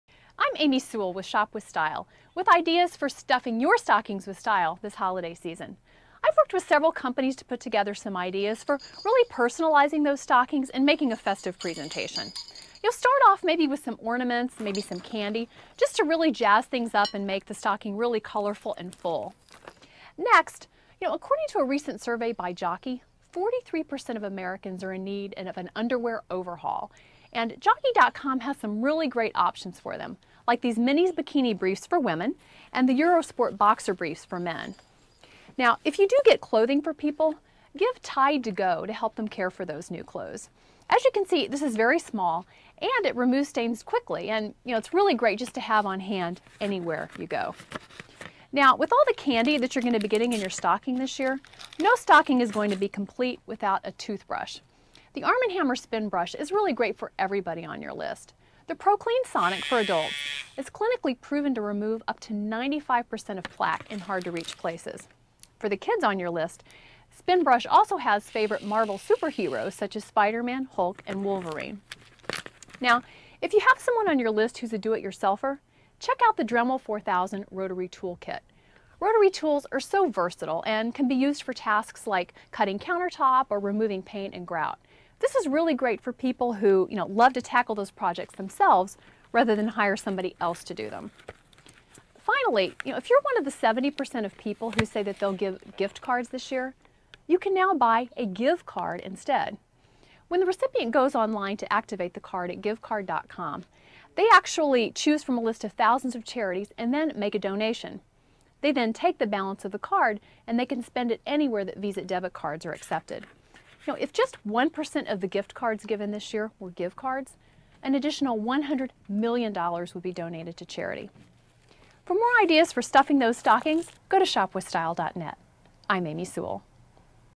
Stuff Your Stockings with Style This Holiday Season!
Amy Sewell, Founder of Shopwithstyle.net shows you how to stuff your stockings with style, with great stocking stuffers for everyone on your list.  Video from Jockey, Arm & Hammer Spinbrush, Tide-to-Go, Dremel and GiveCard.com